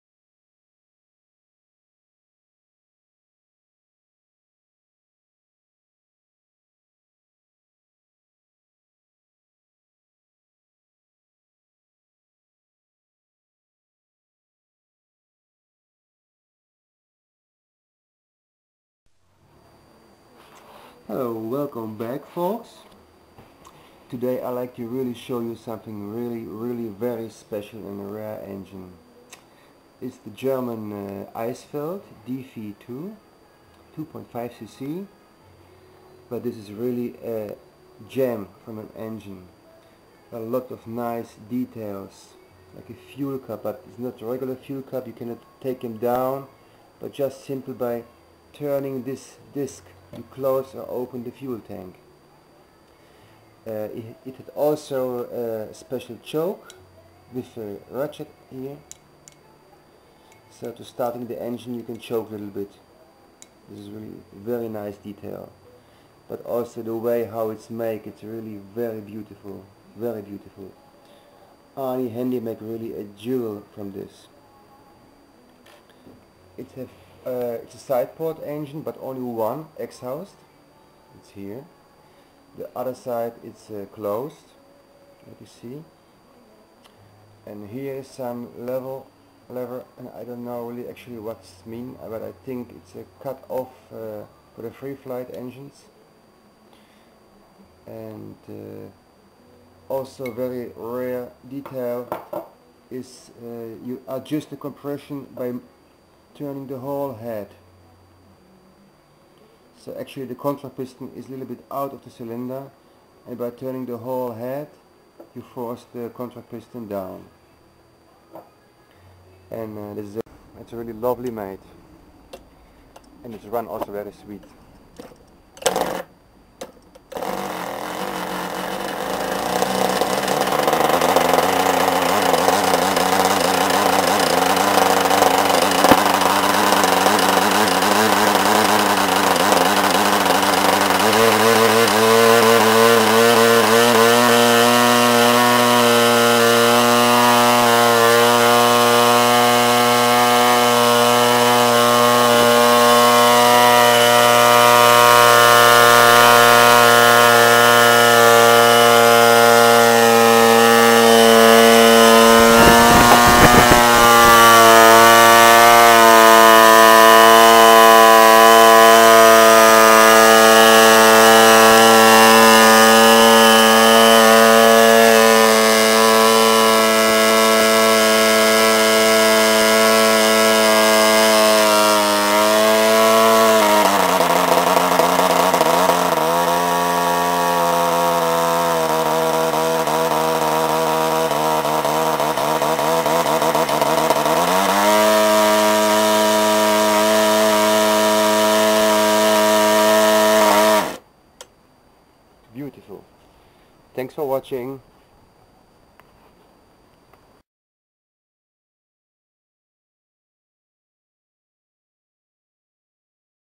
Eisfeld DV2 Modeldiesel 2,5cc. 1943 Arne Hende Copy
Wonderful engine with a lot details,very wel made by repro master himself" Arne Hende".They were different copys made from this lovely old  engine.The Eisfeld engine was the first  model diesel engine sold comercialy in germany.
The special choke,the drain srew, the special filler cap,...some of the lovely details. and a lovely sound of the longstroke sideport design makes it compleet!
Its a nice, but heavy engine,...but who care about the weight on this one,right?
Moore stuff on its way...